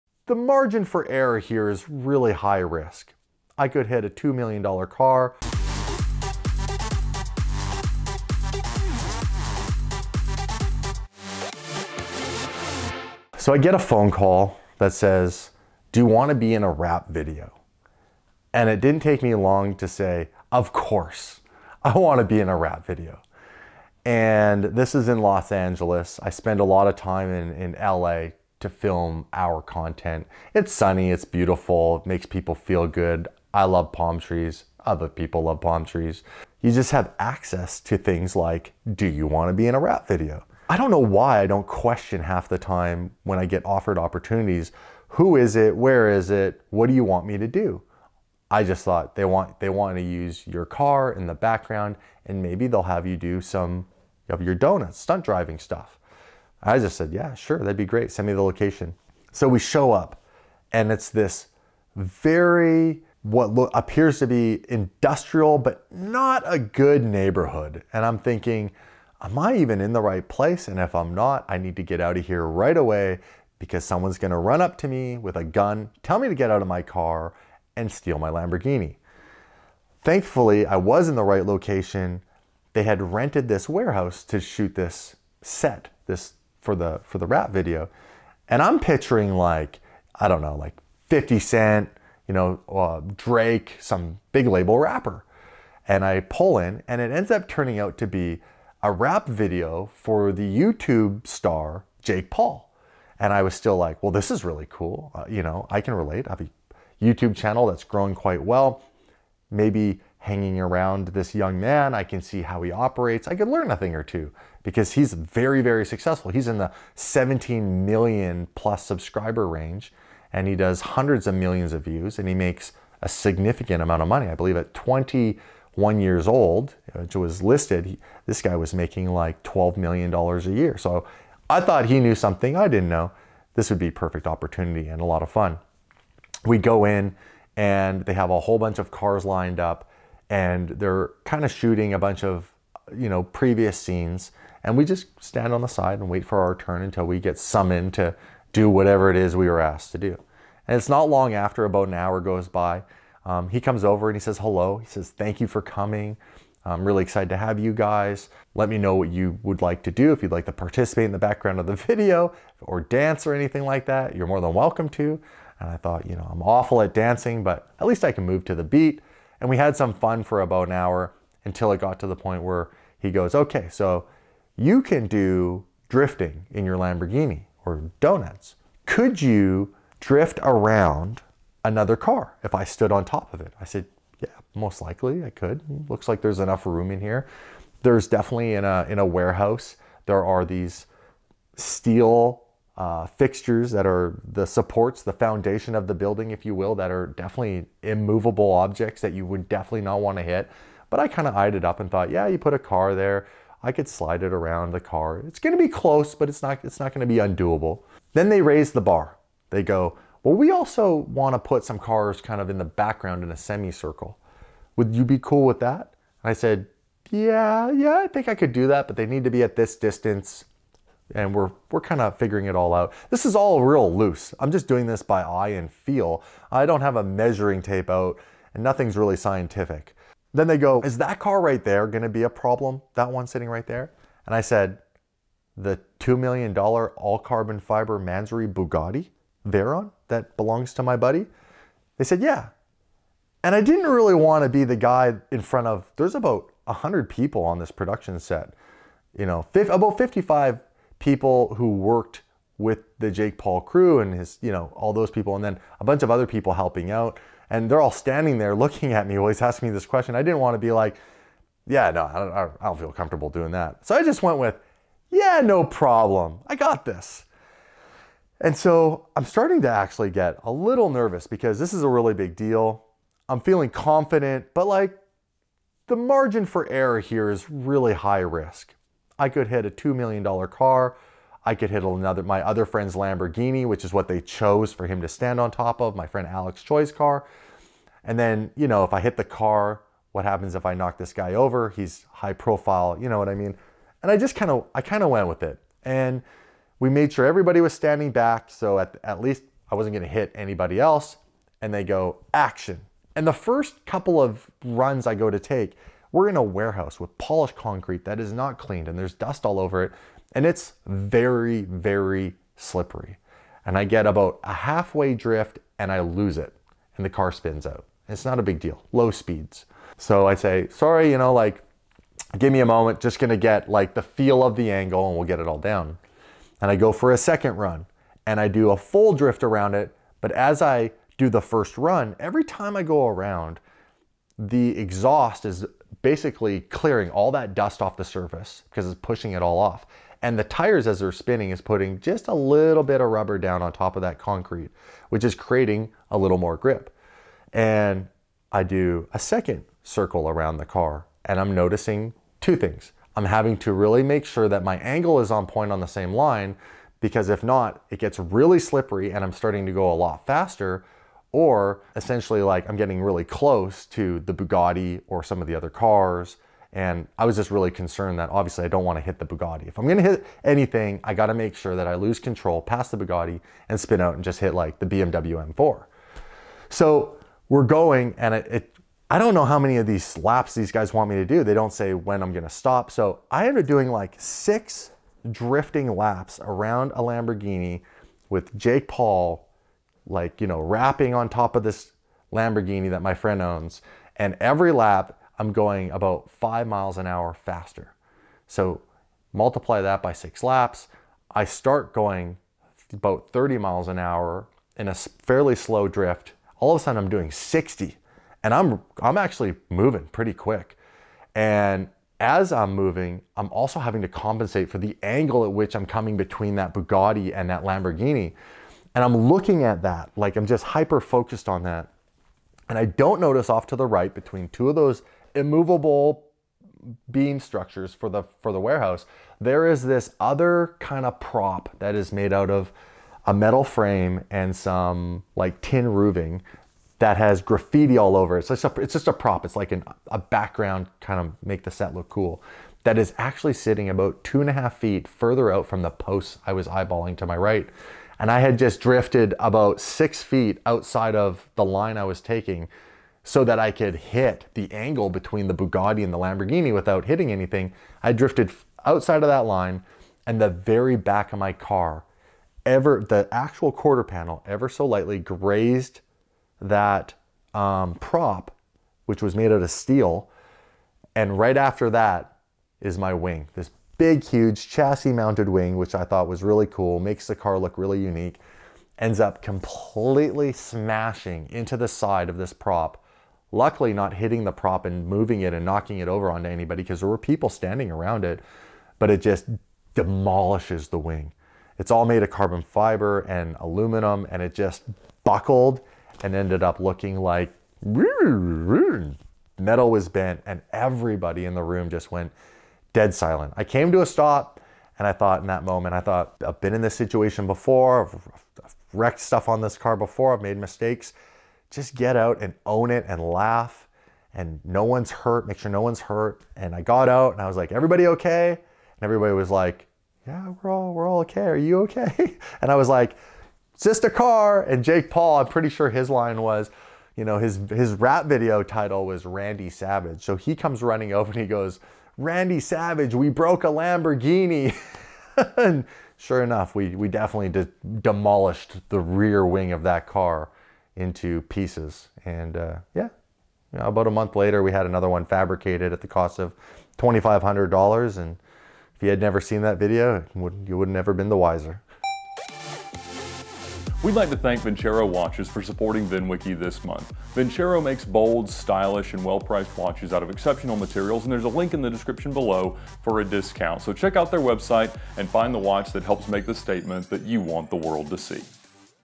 Crashing a Lambo in Jake Paul's Music Video!
Damon from Daily Driven Exotics is always up to something crazy in a car. Sliding a Huracan between Jake Paul and a Bugatti certainly qualifies.

Download VINwiki and add to the stories of the cars you love.